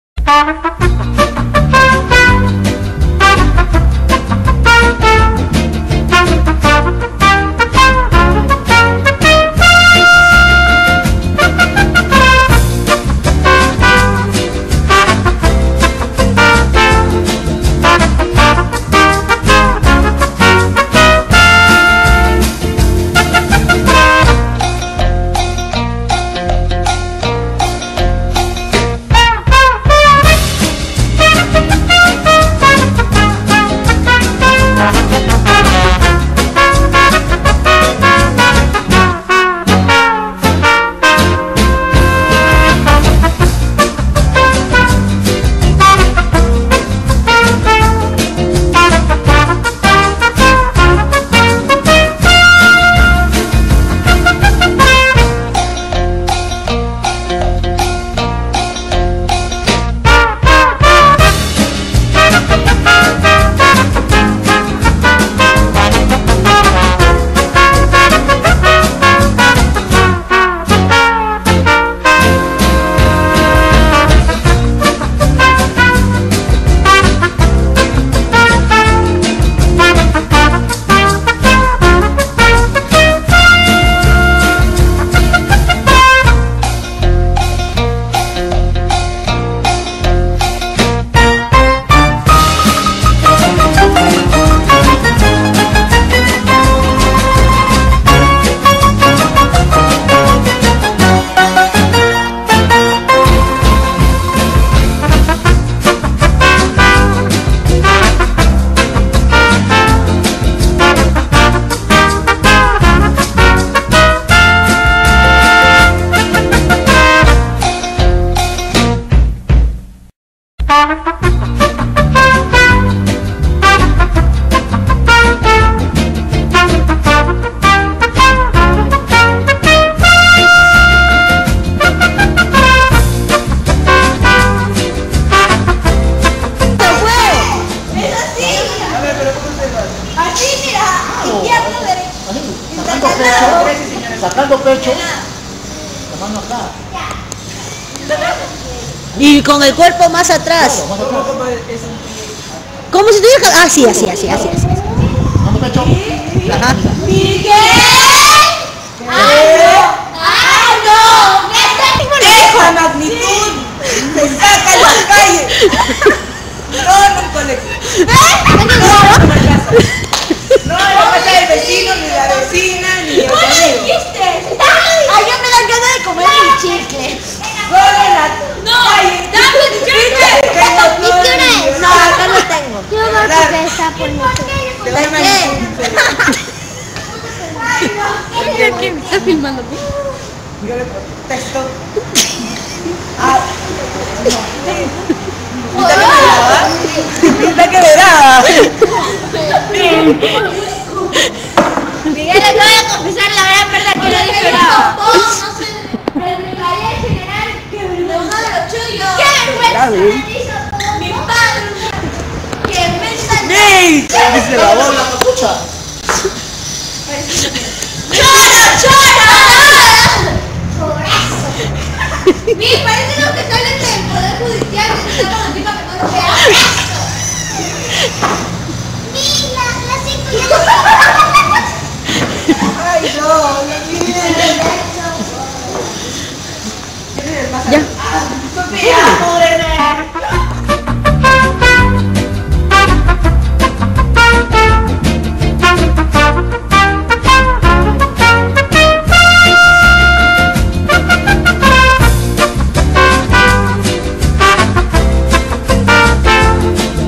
TALLER DE TEATRO JML-FAMILIA MICHIFUZ EXTRACTO de ensayos
NUESTROS ALUMNOS EN SUS ENSAYOS DE VACACIONES PARA LA PRESENTACIÓN DEL FESTIVAL DE TEATRO...